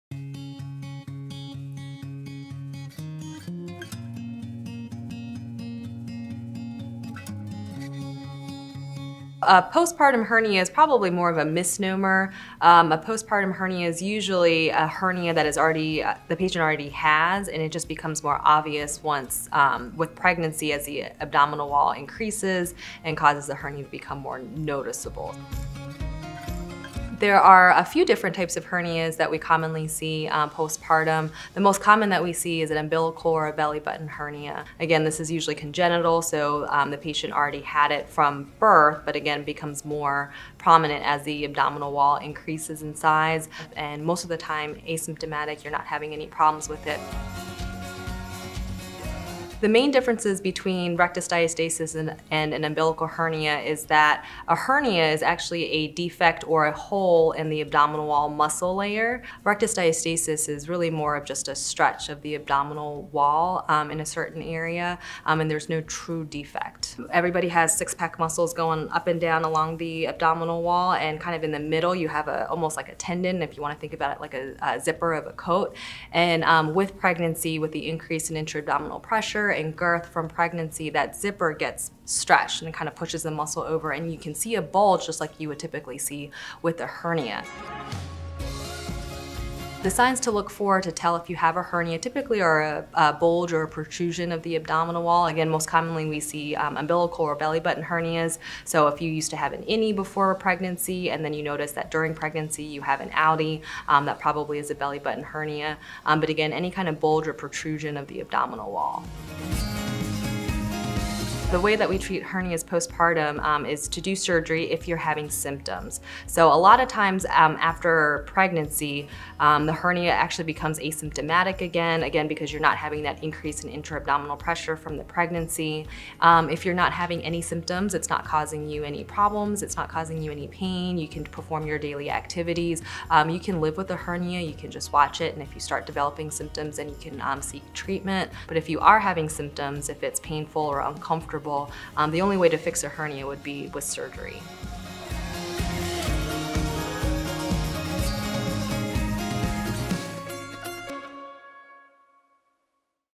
Fast 5: Dr. Ann Chung on Postpartum Hernia
What exactly is a postpartum hernia? Is it any different than a regular hernia? Dr. Ann Chung answers the most common questions about hernias after pregnancy. Visit wakemed.org to learn more about the comprehensive services offered by WakeMed General Surgery.